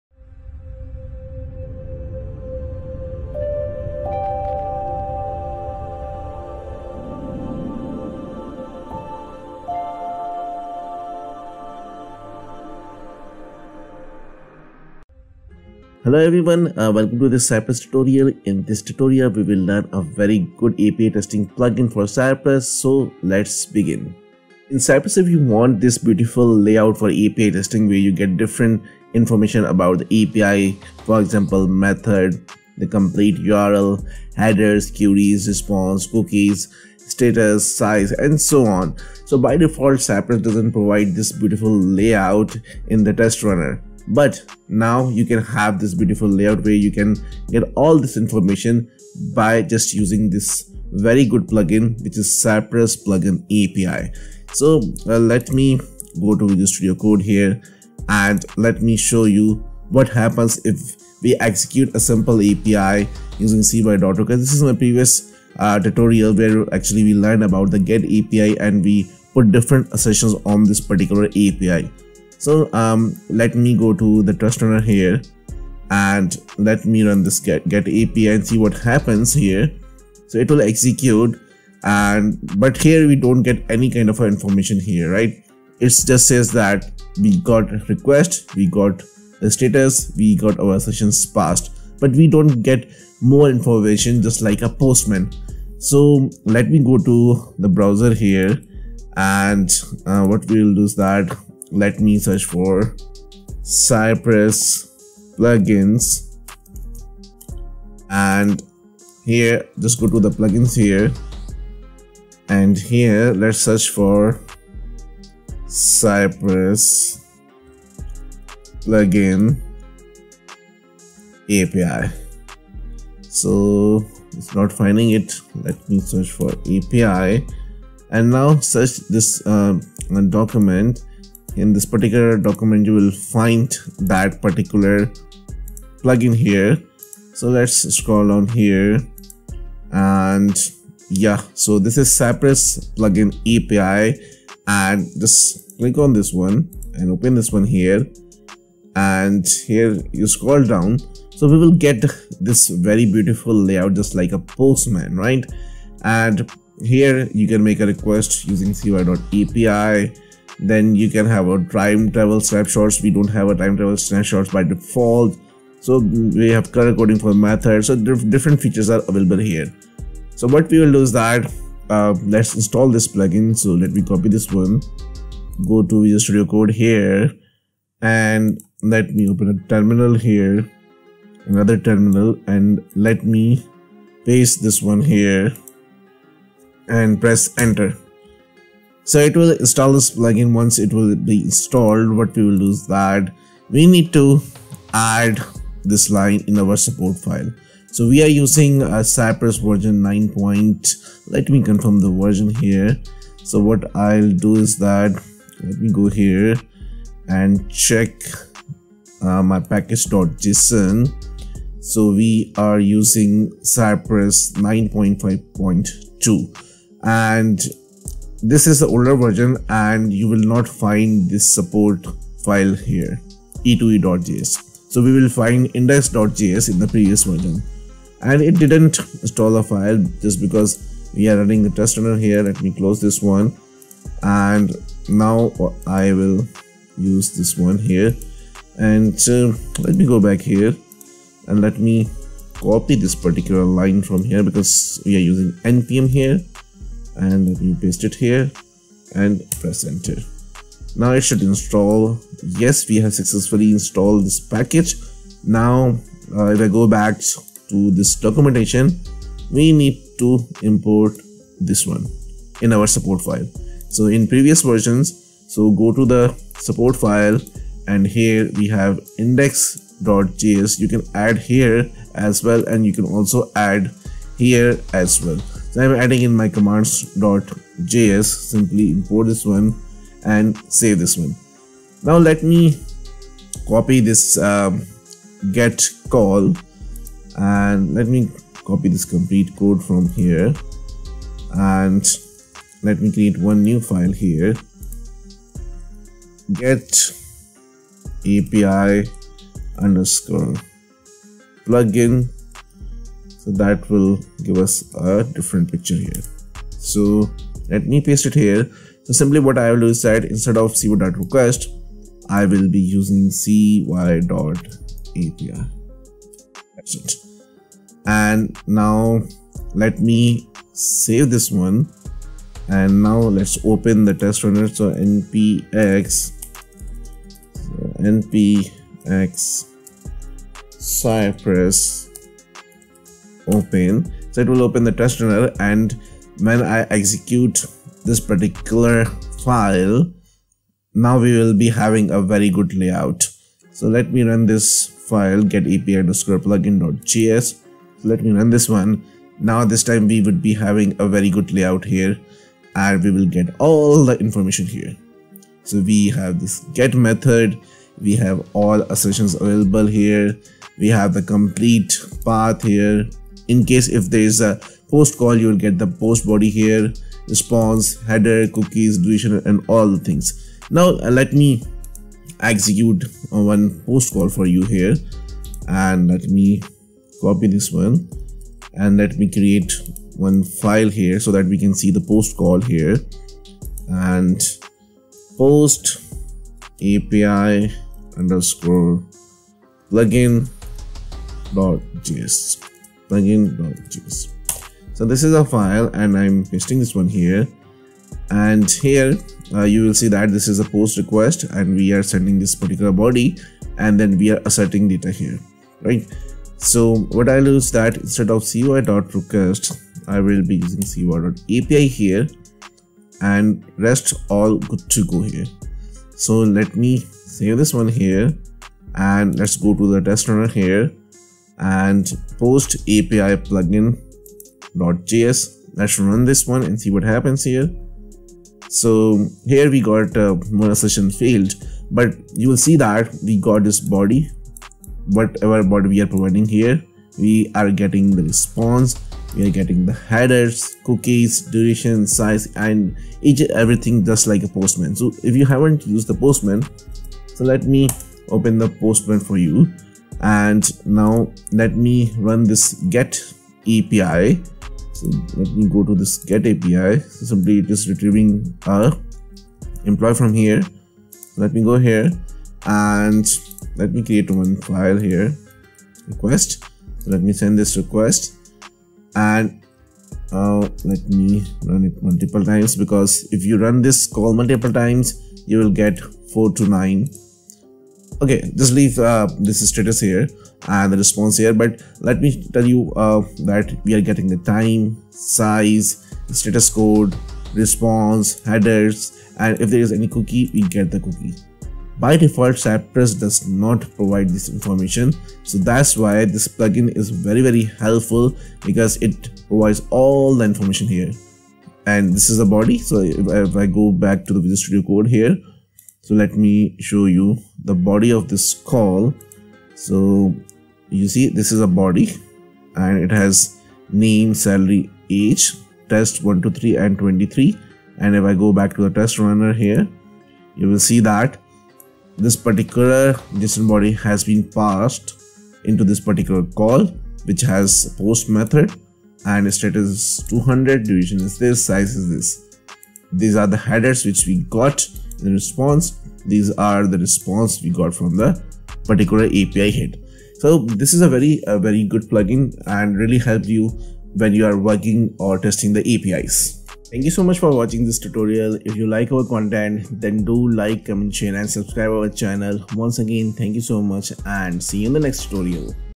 Cypress tutorial #34 | API Testing Plugin for Cypress
- Introduction to Cypress API Plugin
- How to download the cypress API Plugin
- How to configure cypress API Plugin
- How to use cypress API Plugin
- How to use cy.api in cypress
- How to use cy.api using cypress API plugin
- Example of cypress API plugin
- Example of cypress cy.api
- Example of cypress API plugin tutorial